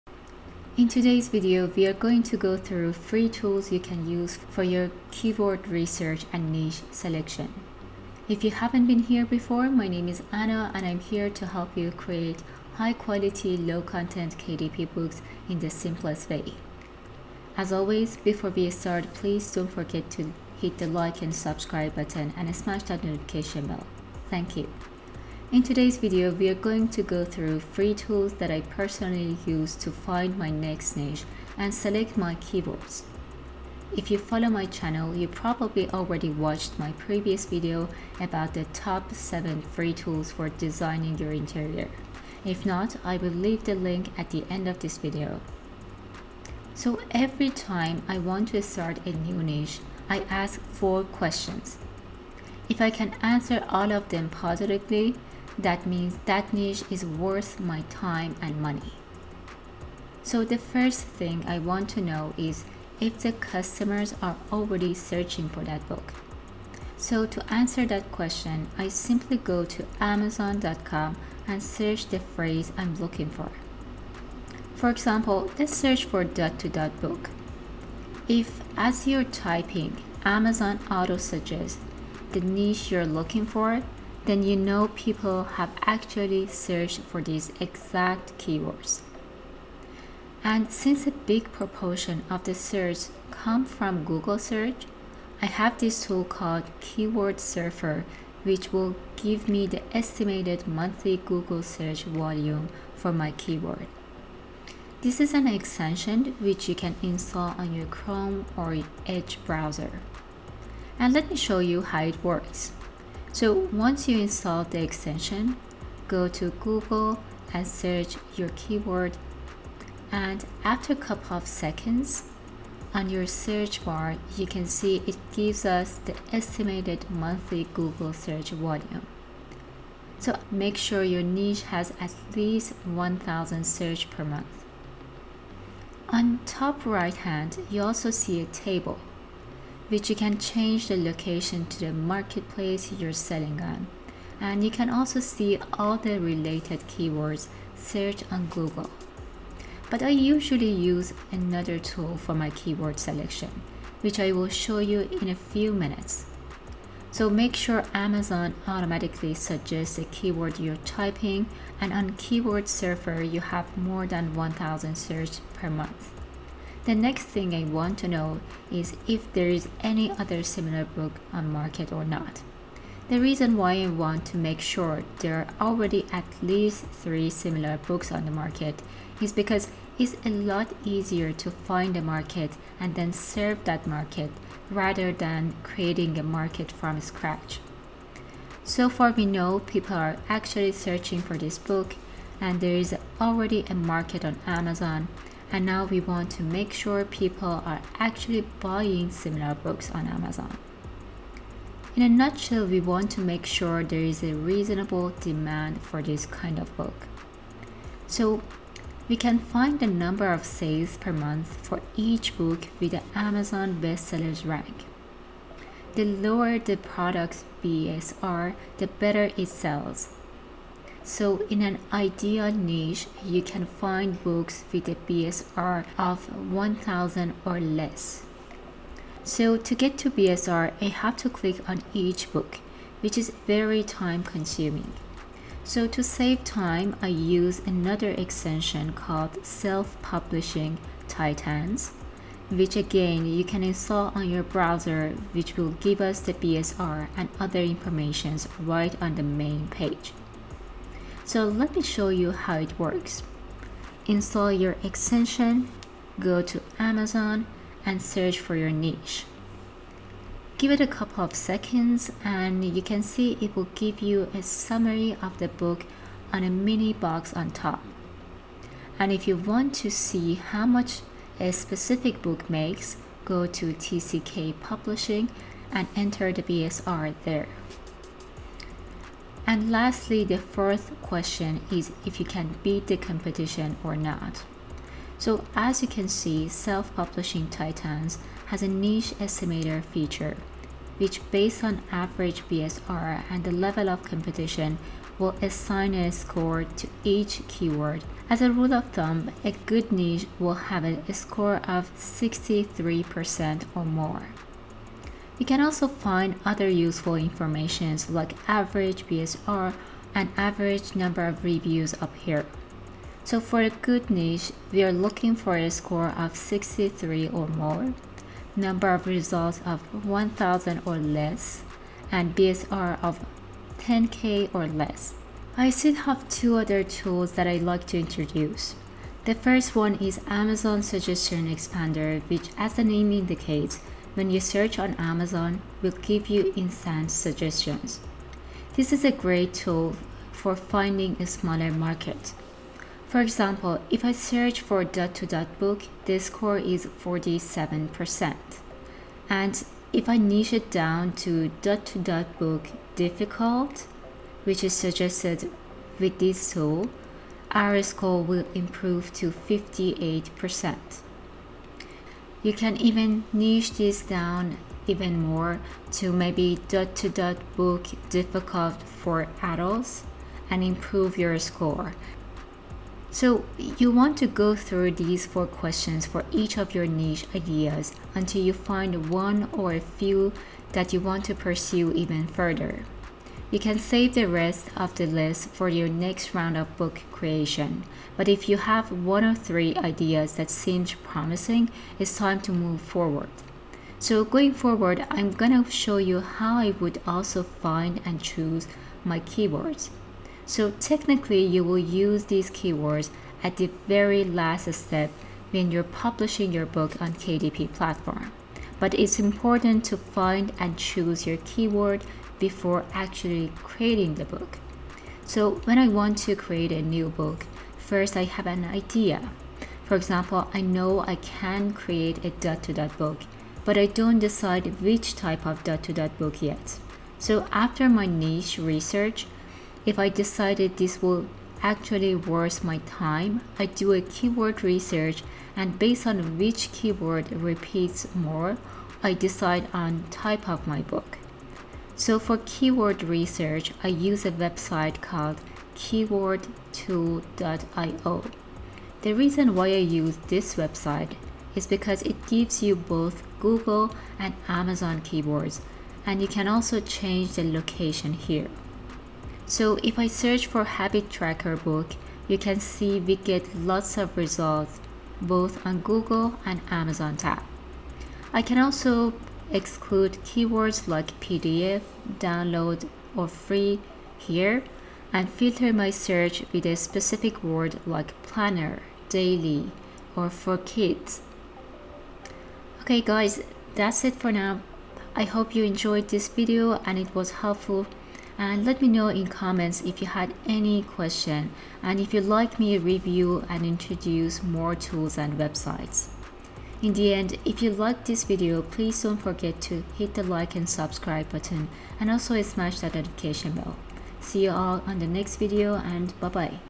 Free tools for niche and keyword research - amazon KDP
In today's video, we are going to go through free tools that I personally use to find my next niche and select my keywords. If not I will leave the link at the end of this video.
So every time I want to start a new niche I ask 4 questions if I can answer all of them positively that means that niche is worth my time and money.

📌Helpful websites:
Drawing tablet